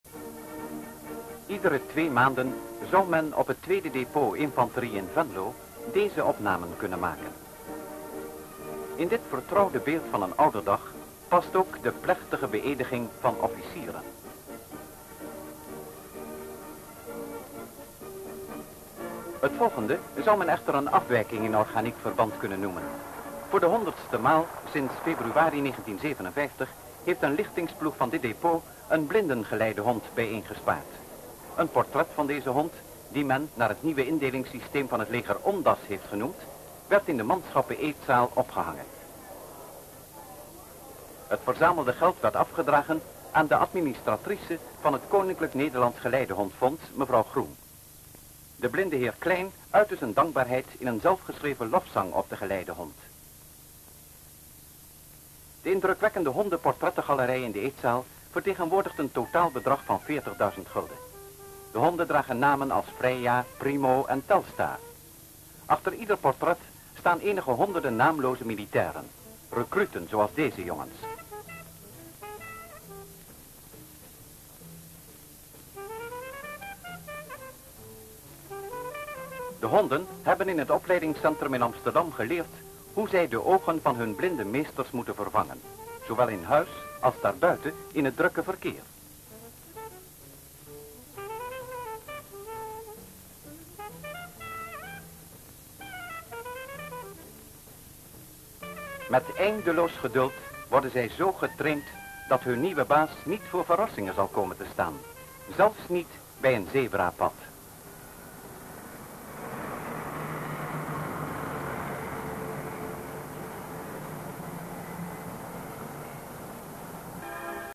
Aanbieding van een blinden geleidehond in Venlo
Aanbieding van een blinden geleidehond door het 2e depot Venlo in 1963.